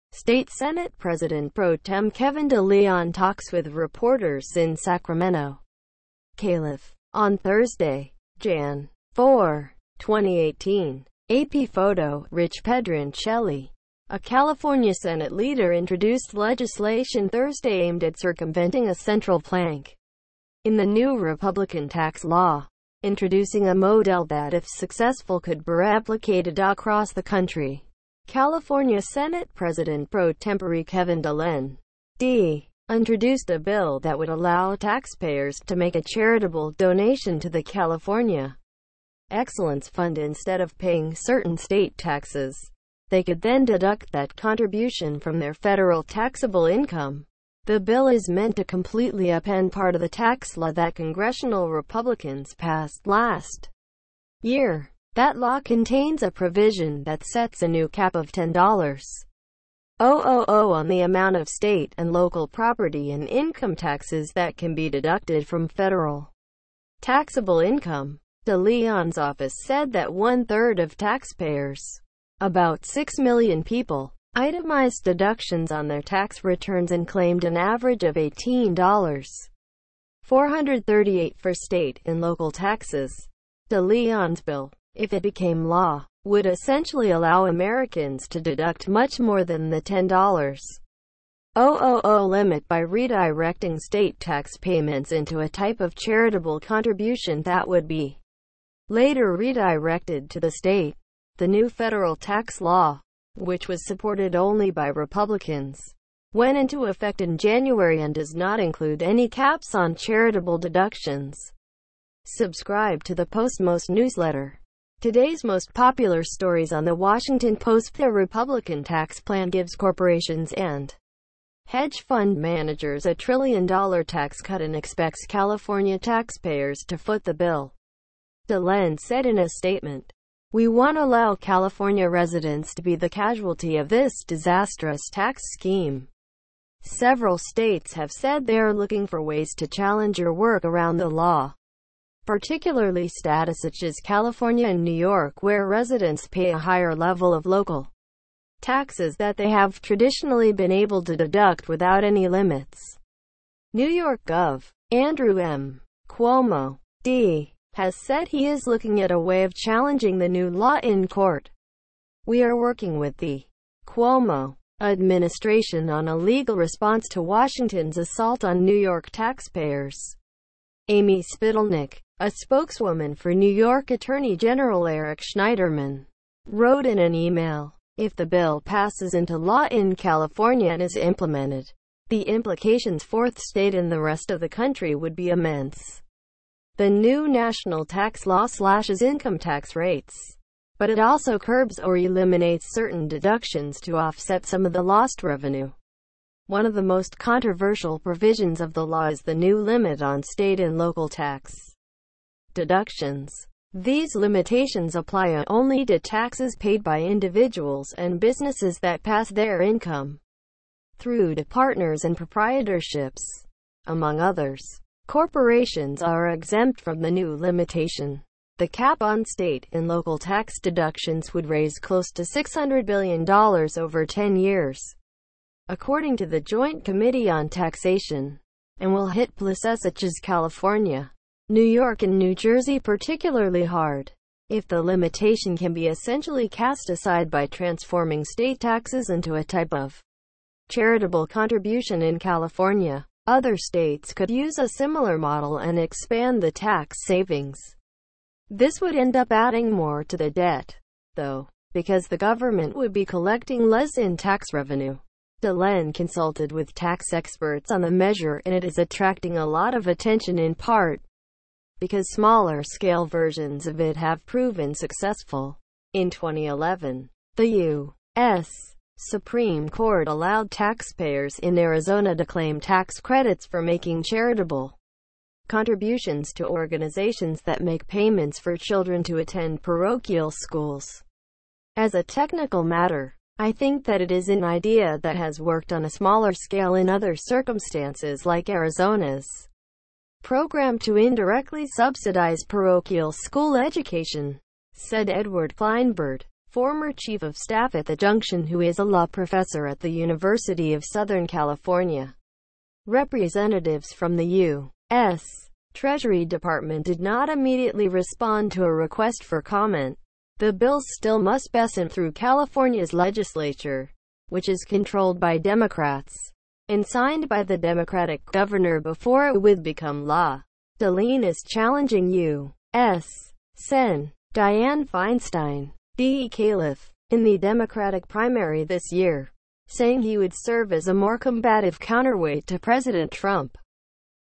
New California bill could serve as national boilerplate for skirting Trump"s tax law
State Senate President Pro Tem Kevin de Leon talks with reporters in Sacramento, Calif. , on Thursday, Jan.  4, 2018.  (AP Photo/Rich Pedroncelli)A California Senate leader introduced legislation Thursday aimed at circumventing a central plank in the newRepublican tax law, introducing a modelthat  if successful  could bereplicatedacross the country. California Senate President Pro Tempore Kevin de Len (D) introduced a bill that would allow taxpayers to make a charitable donation to the California Excellence Fund instead of paying certain state taxes.  They could then deduct that contribution from their federal taxable income.